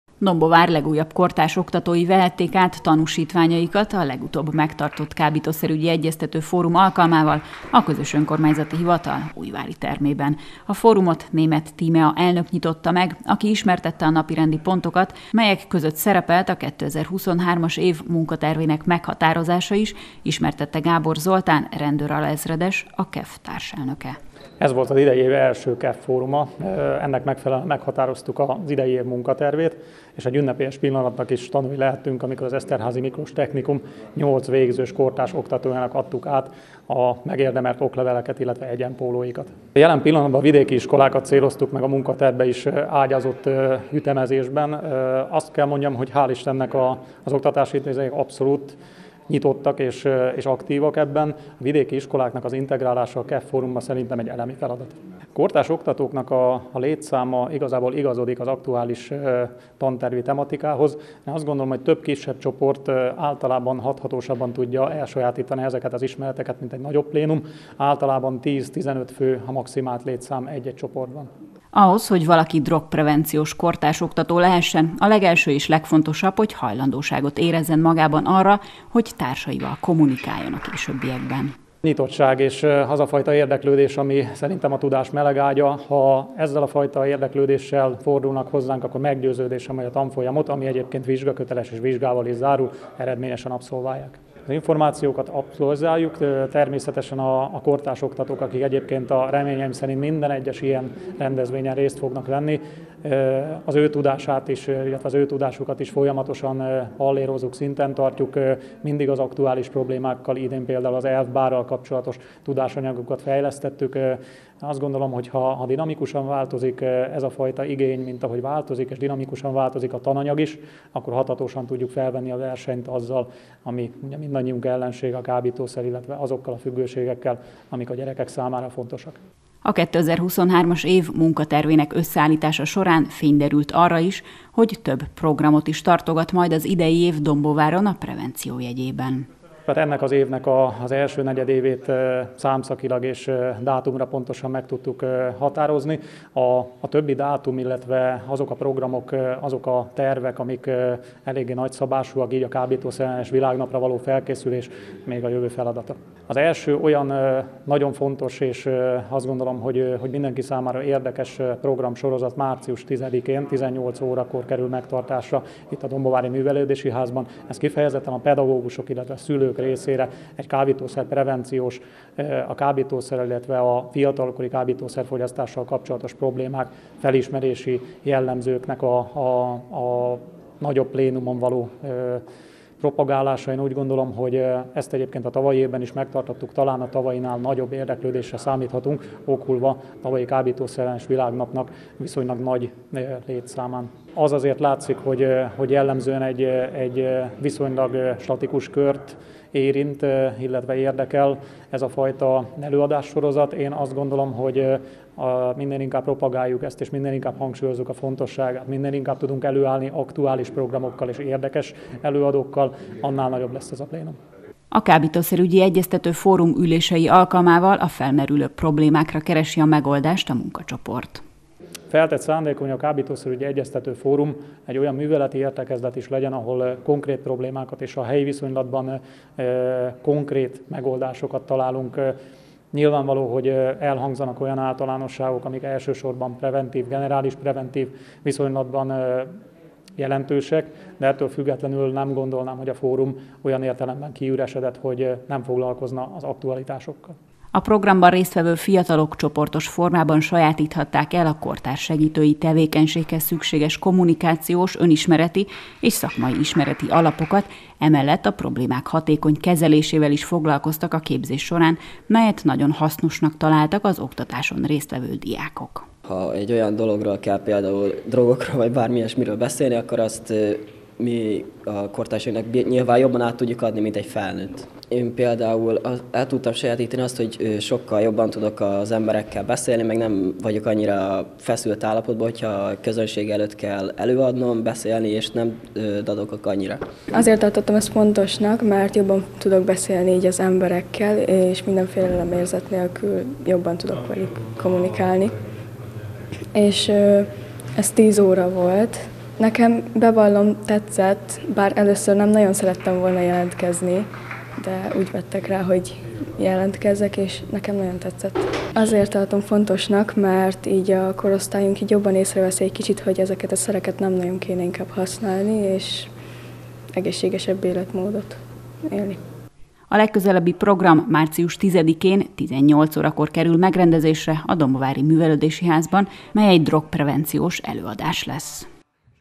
Kortárs oktatók képzése - Kábítószerügyi Egyeztető Fórum, Dombóvár
Megtartotta idei első ülését a Dombóvári Kábítószerügyi Egyeztető Fórum. A 2023. évi munkaterv meghatározása során szó esett a kortárs oktatók további képzéséről, a KEF bővítéséről a vidéki iskolák irányába, a pályázati források kiaknázásáról, valamint nem utolsó sorban a drogprevenciós előadások megszervezéséről.